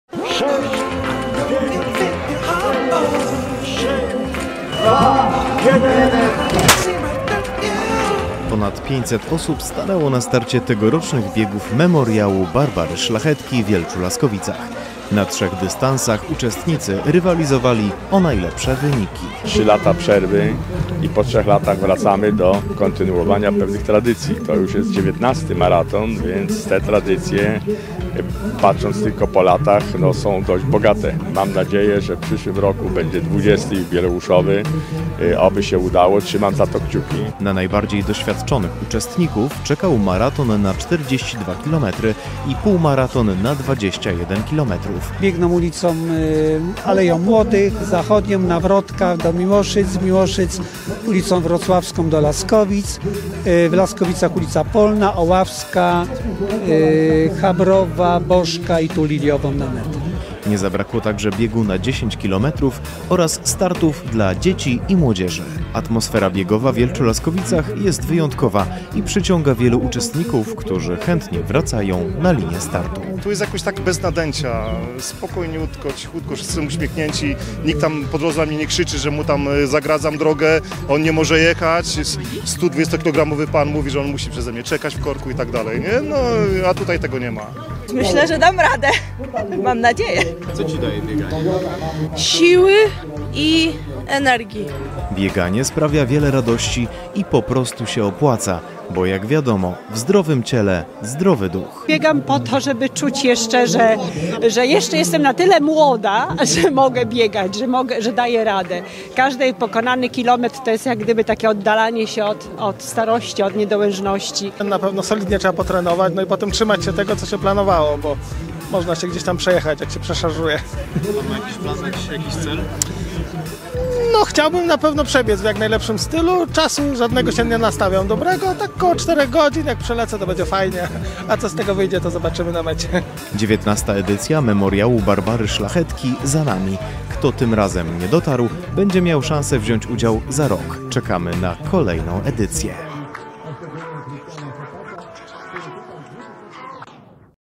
XIX Toyota Maraton Jelcz-Laskowice! 2023
Ponad 500 osób stanęło na starcie tegorocznych biegów Memoriału Barbary Szlachetki w Jelczu-Laskowicach. Na 3 dystansach uczestnicy rywalizowali o najlepsze wyniki.